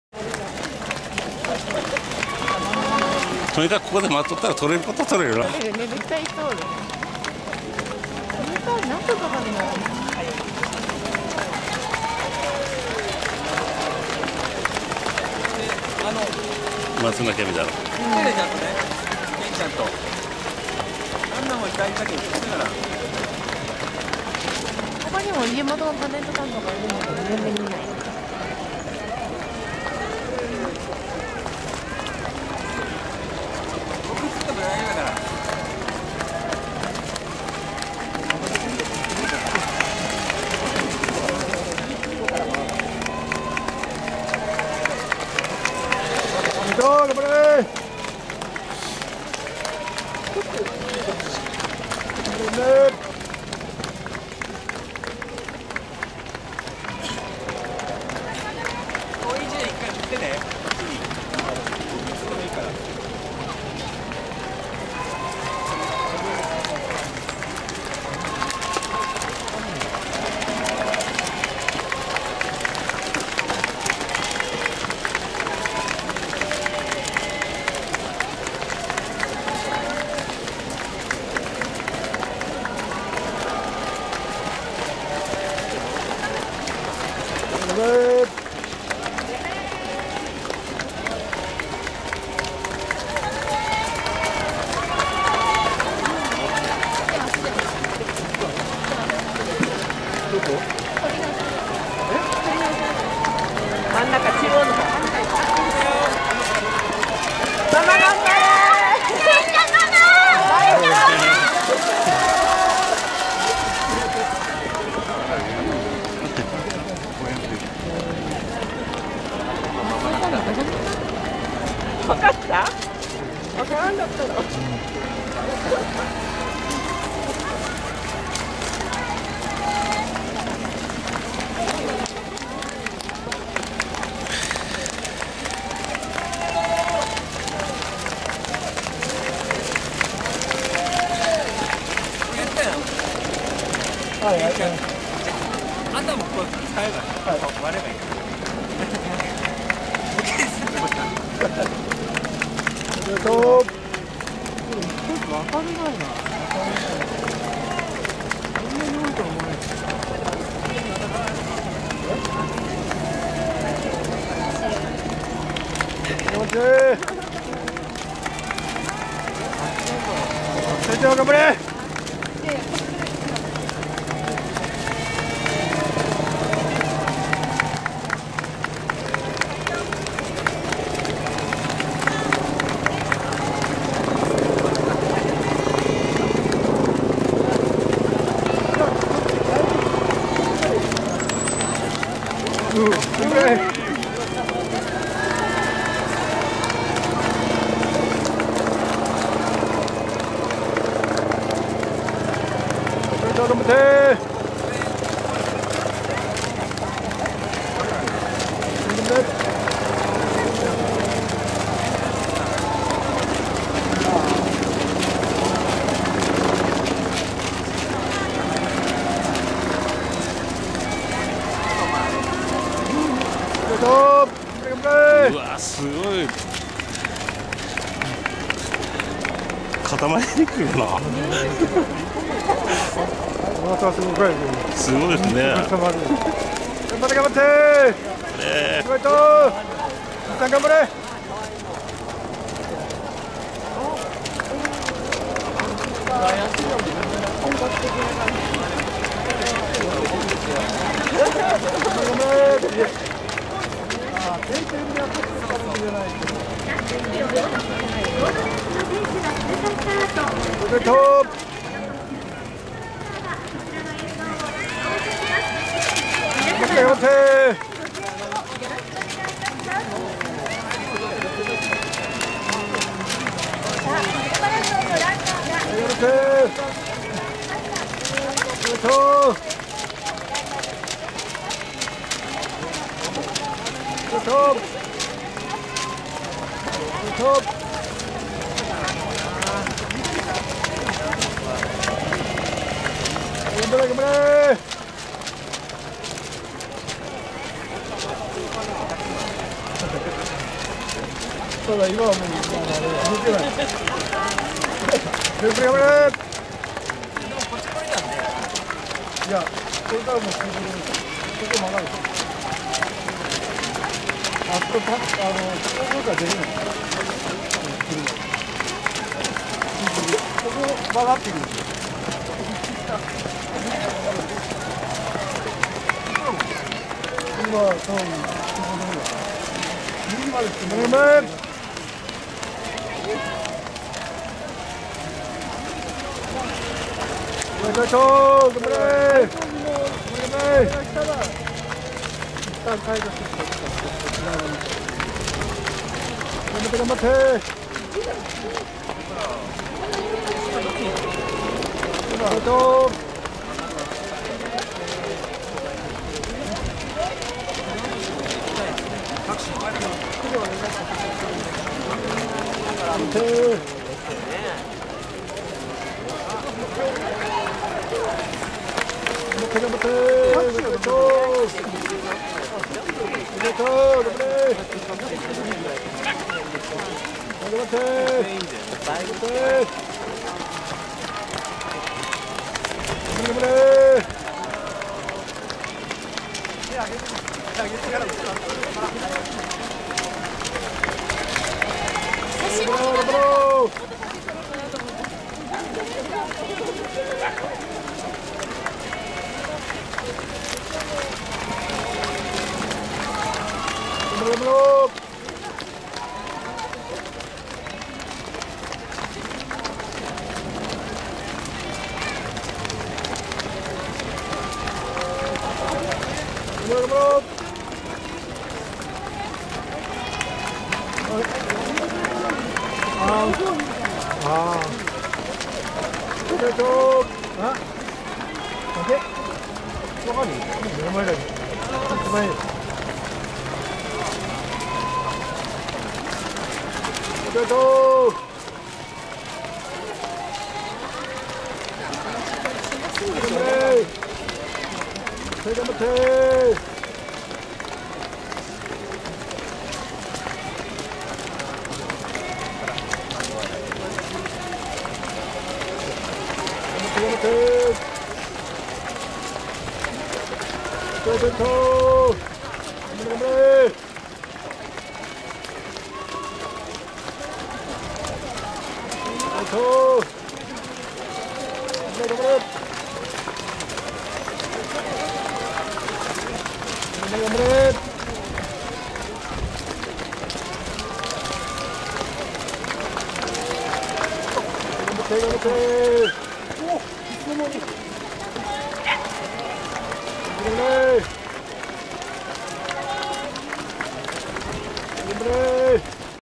第一回熊本城マラソン10km地点 2/7 松野明美さん家族声かけシーン収録
第一回熊本城マラソン(フルマラソン)10km地点 2/7 松野明美さん付き
松野明美さんが家族に向けて声かけしているシーンを収録
熊本西大橋〜アクアドーム前T字路交差点前
県道277号線_県道51号線 交差点
2012年2月19日午前9時44分〜9時52分頃